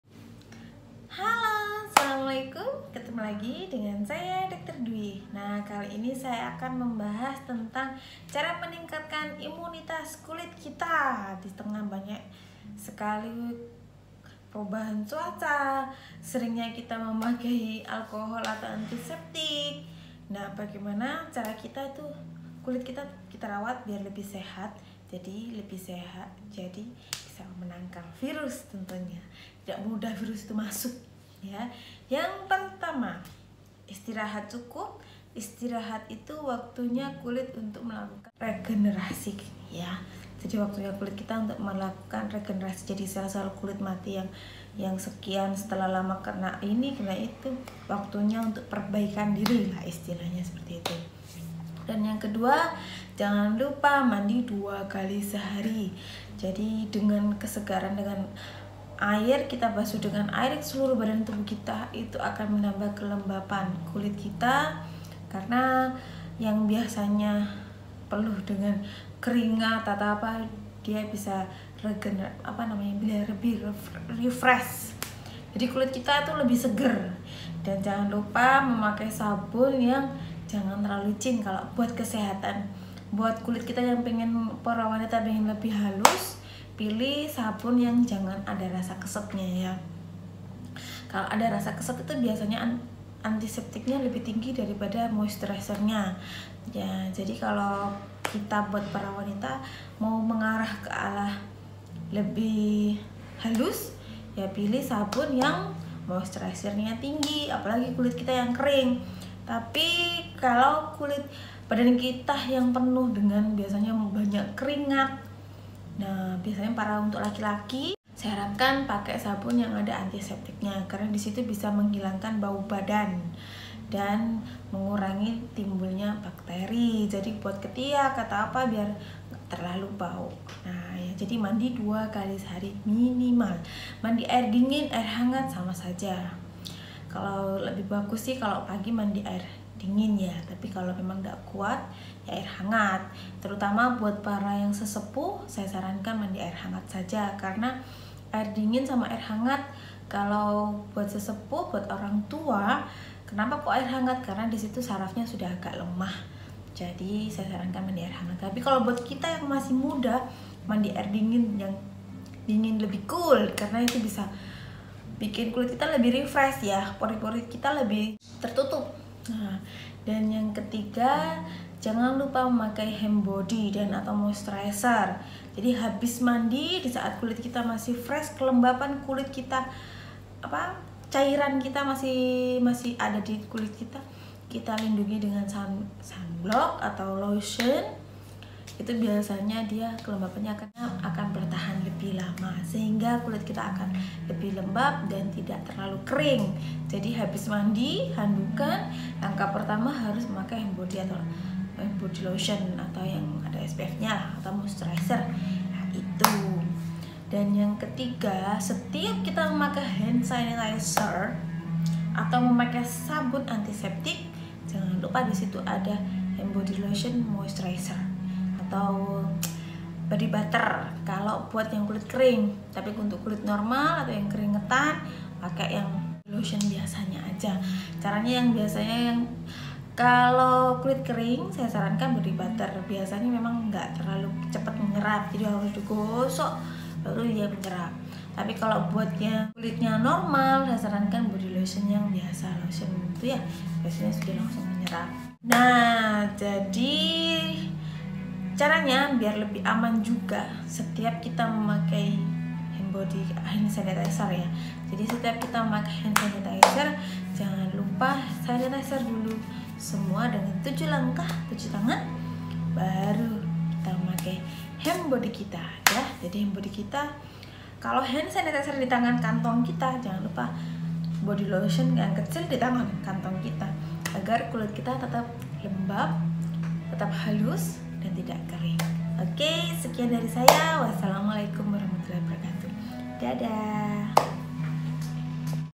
menjaga kualitas kulit
Meningkatkan imunitas supaya tidak mudah sakit dan kulit terjaga